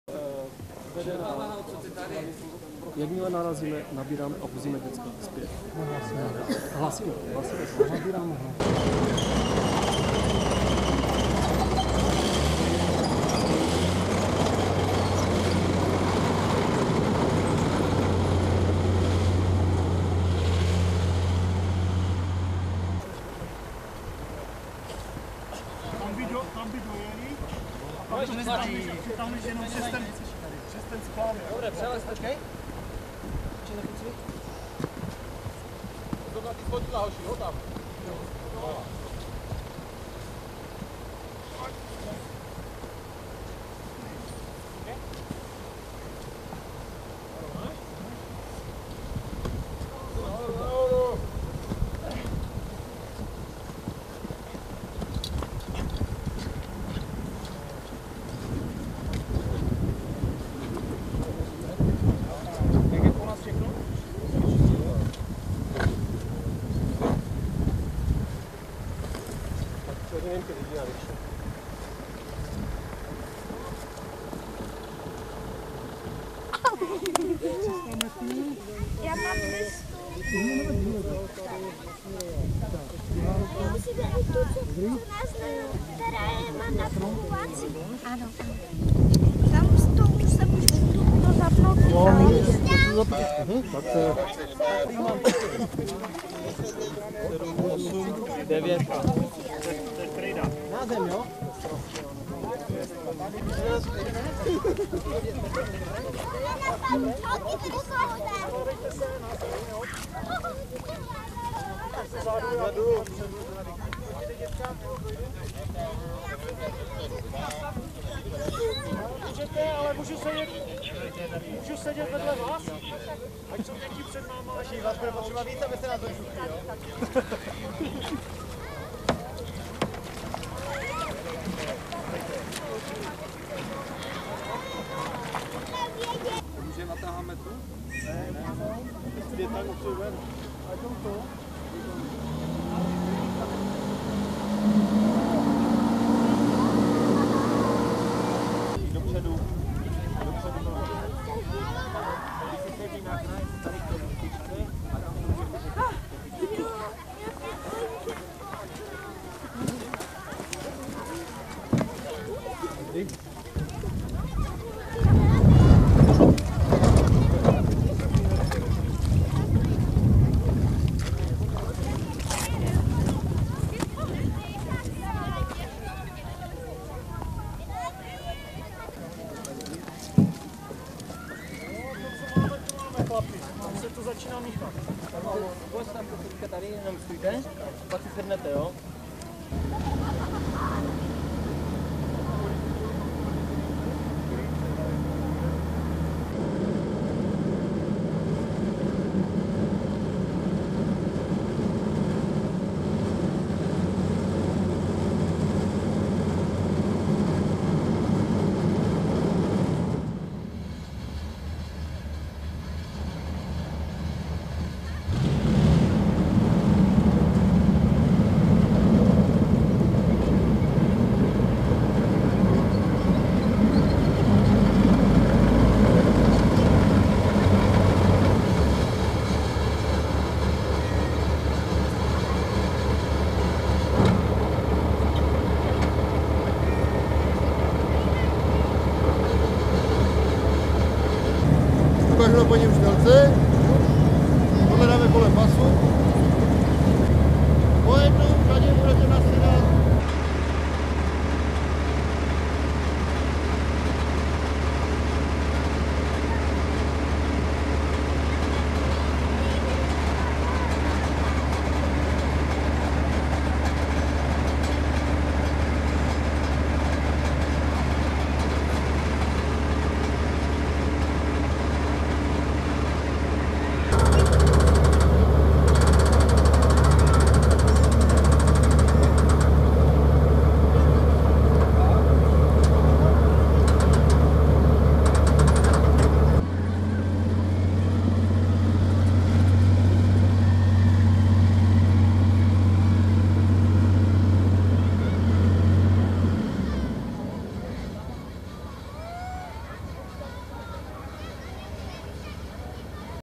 Těrlicko: Cvičení evakuace 300 školáků z okolí přehrady
Jedno z největších cvičení složek Integrovaného záchranného systému se konalo v pátek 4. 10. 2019 na Těrlické přehradě a v jejím okolí. Motivováno bylo následky bleskových povodní, po kterých na různých vyvýšených místech zůstalo uvězněno přes 300 školáků. Hasiči měli za úkol je najít a  s pomocí člunů a obojživelníků evakuovat do bezpečí.